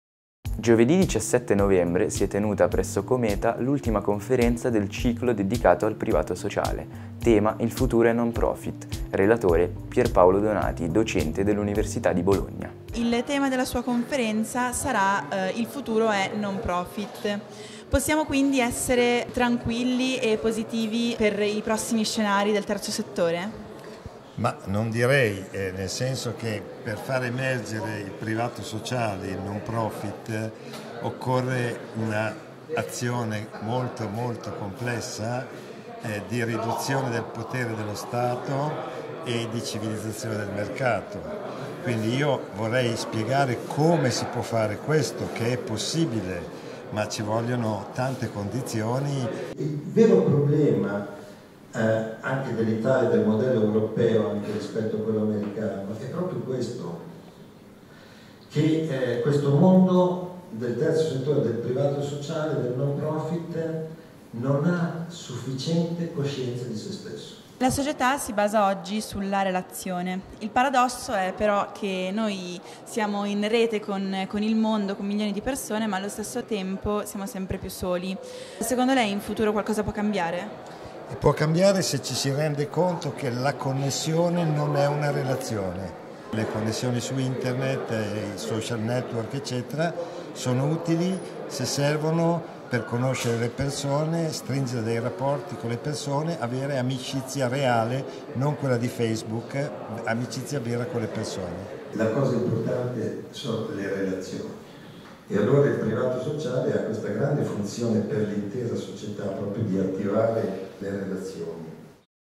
Il futuro è non profit. Intervista a Pierpaolo Donati
Intervista a Pierpaolo Donati in occasione della conferenza organizzata il 17 novembre dalla Fondazione Comasca dal titolo "Il futuro è non profit". Il prof. Donati spiega il ruolo che dovrebbe avere il privato sociale nella nostra società, le sfide che deve superare in un mondo sempre più connesso ma sempre più disaggregante.